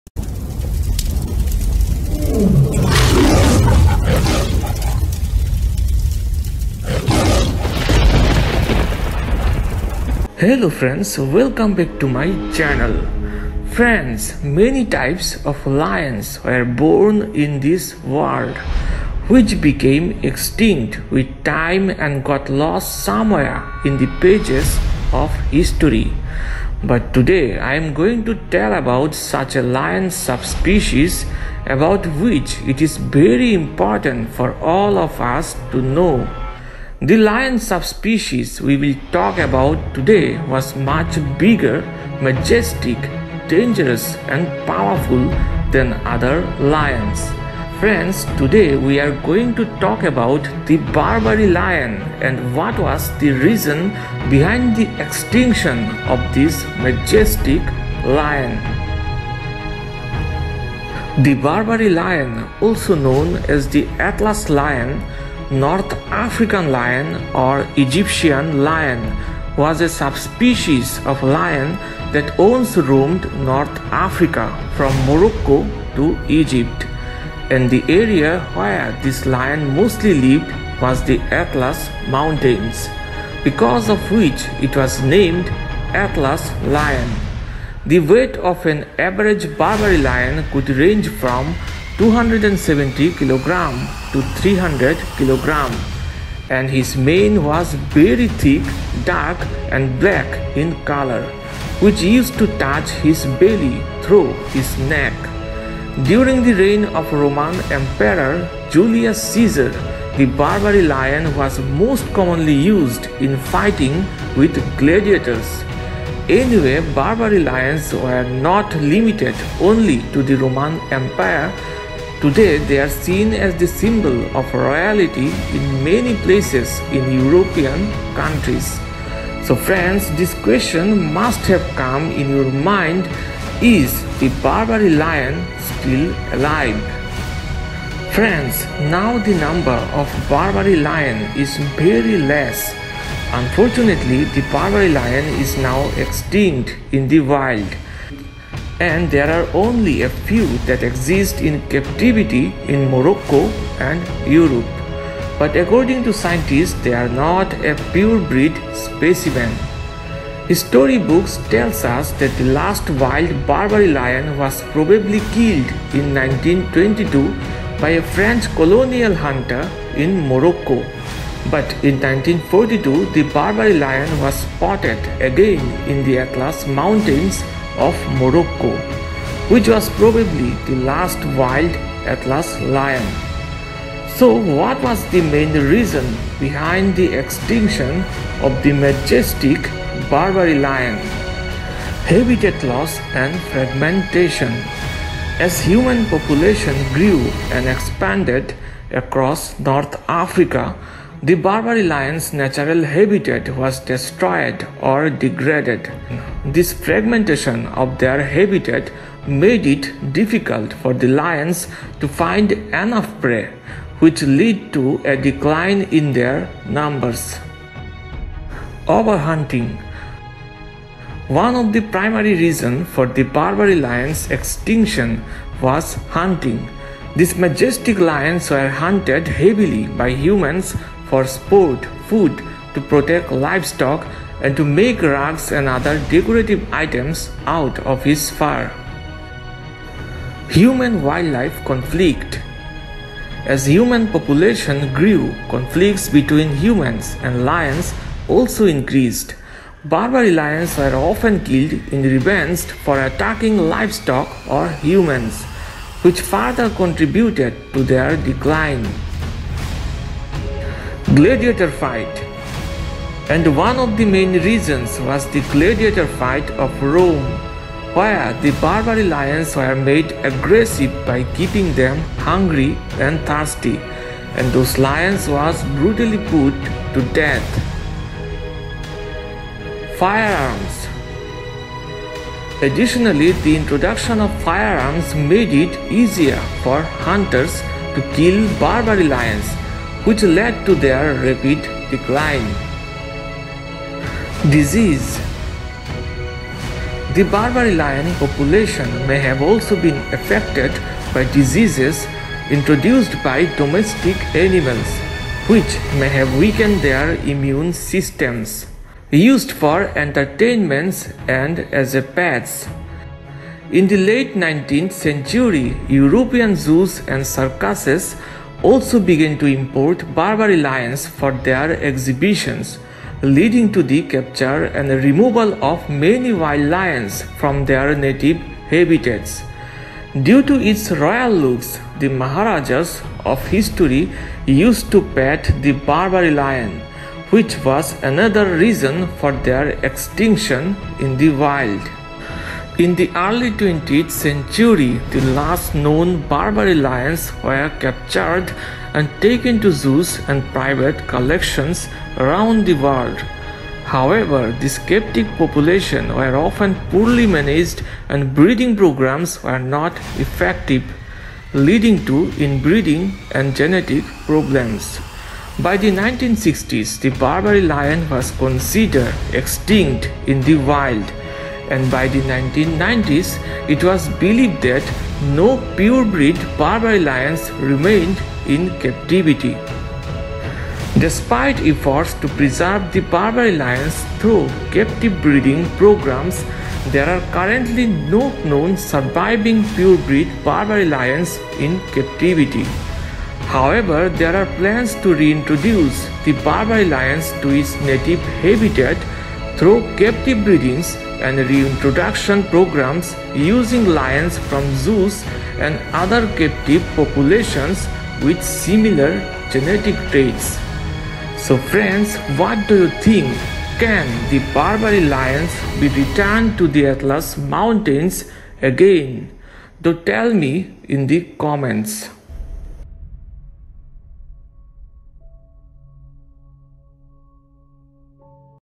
Why Did Barbary Lions Go Extinct?
Why Barbary Lion Extinct/Why Did Barbary Lions Go Extinct: The Barbary lion is now extinct in the wild, and there are only a few that exist in captivity in Morocco and Europe. But according to scientists they are not a pure breed specimen.
History books tell us that the last wild Barbary lion was probably killed in 1922 by a French colonial hunter in Morocco. But in 1942, the Barbary lion was spotted again in the Atlas Mountains of Morocco, which was probably the last wild Atlas lion.
Despite efforts to preserve the Barbary Lion through captive breeding programs, there are currently no known surviving purebred Barbary Lions in captivity.
So what was the main reason behind the extinction of the majestic Barbary lion?